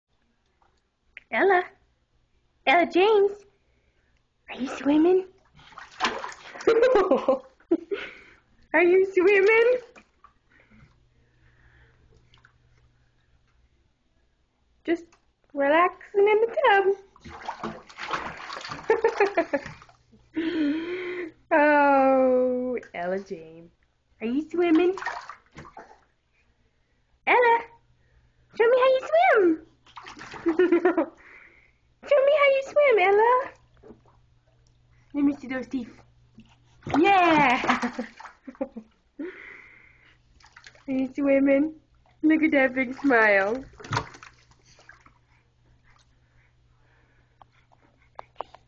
ella james bath
i figured i'd give sister a bath too!
she just needs a pina colada in her hand.
she LOVES the water